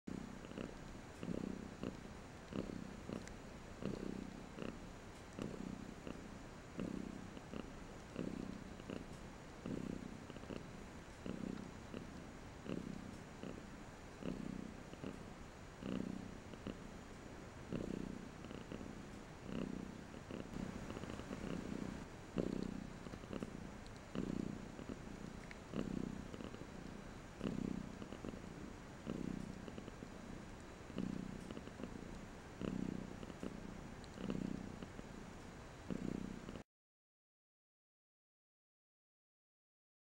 Purrs from Wall e the cat - cat's strong purrs
Volume up to hear her strong purrs :)